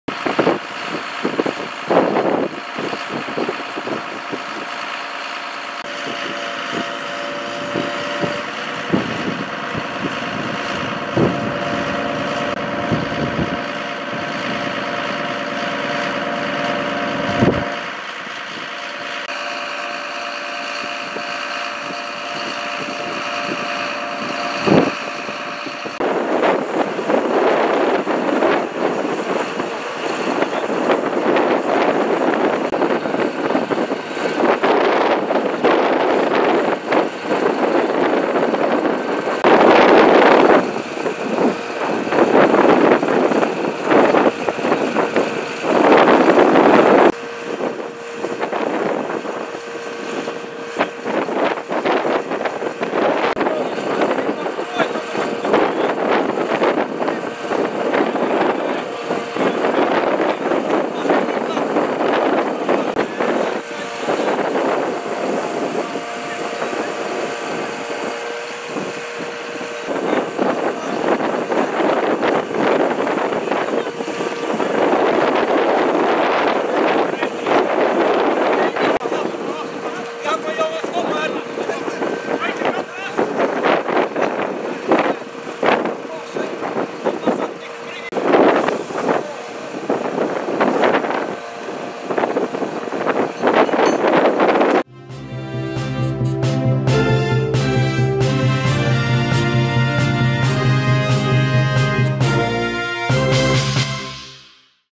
Brandweer haalt met flink wat moeite paard uit de sloot
Sint-Maartensdijk - Dinsdagmiddag is de brandweer van Sint-Maartensdijk en Sint-Annaland uitgerukt voor een paard in de sloot langs de Provincialeweg tussen Sint-Maartensdijk en Stavenisse. Het paard is met behulp van de kraan van het hulpverleningsvoertuig uit de sloot getild.